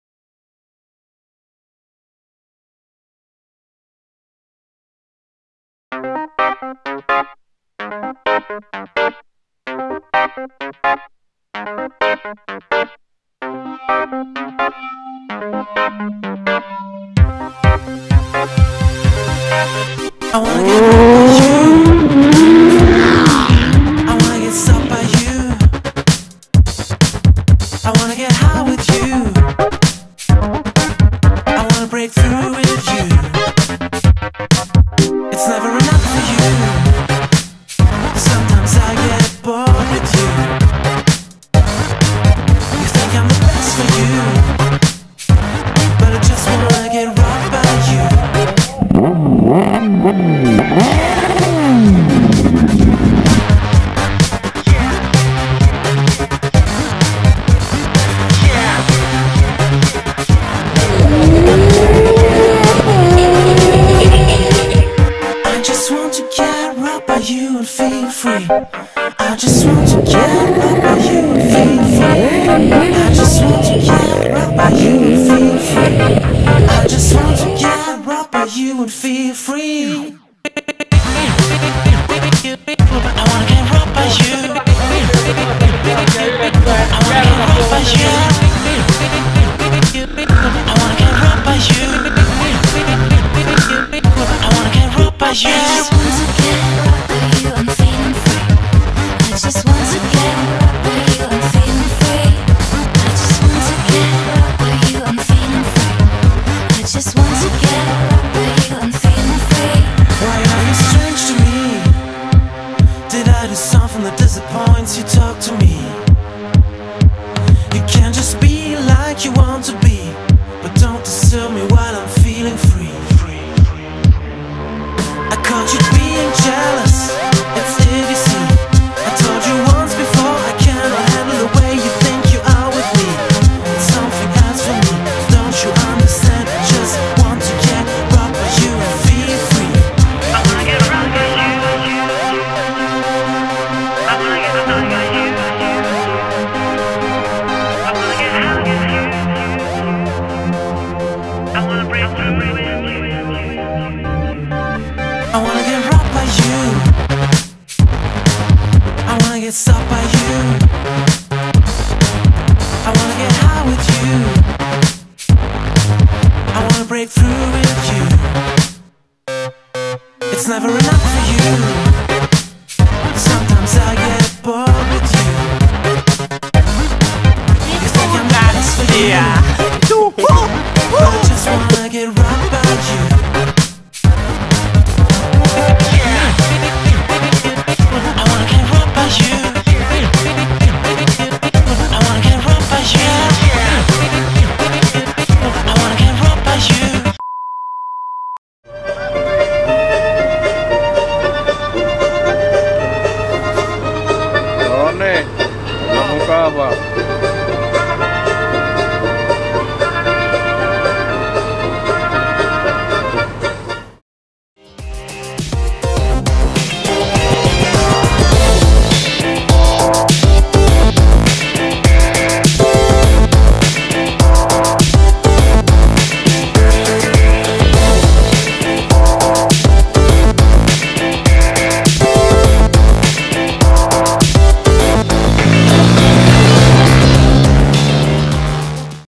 TRR 2014 - Episode 2/10 - Vaakuna Rally show
Our show from Vaakuna rally: wheelies on snow, drifting with a limousine